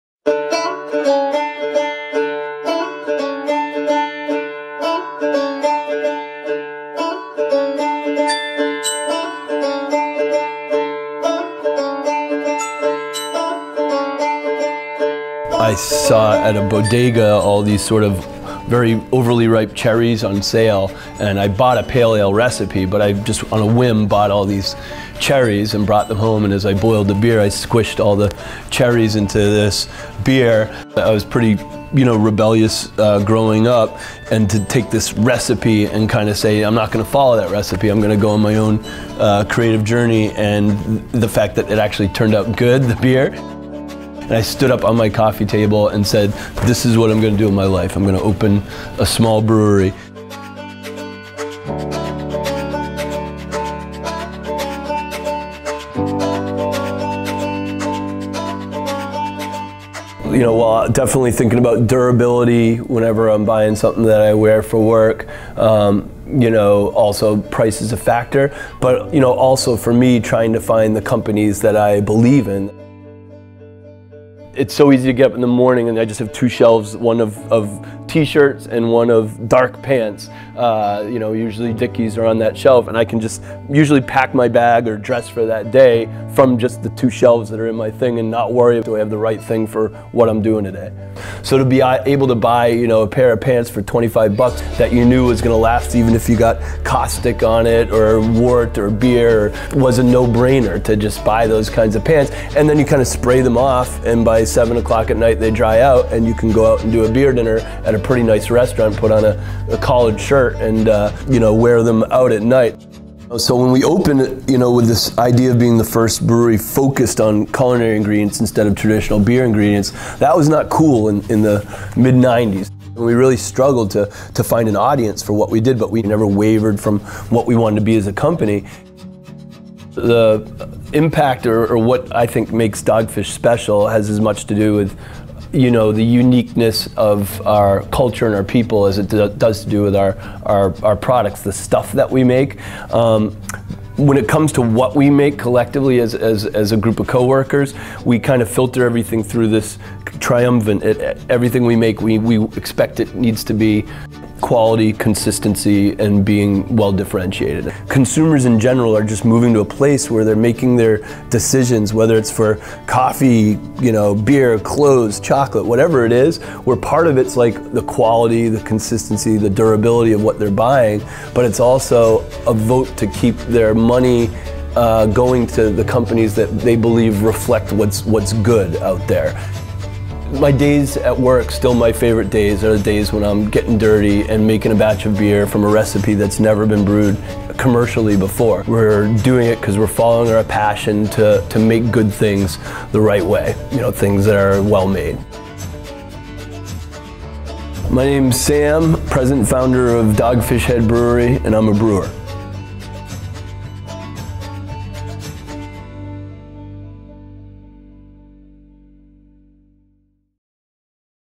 Dickies Dogfish Head Brewery
Sam Calagione, owner and founder of Dogfish Head Brewery knows it takes great workwear to make a great brew. He relies on Dickies to provide superior workwear that can withstand any environment he and his team experience, both on and off the job. To learn more about Dogfish Head Brewery and Dickies Workwear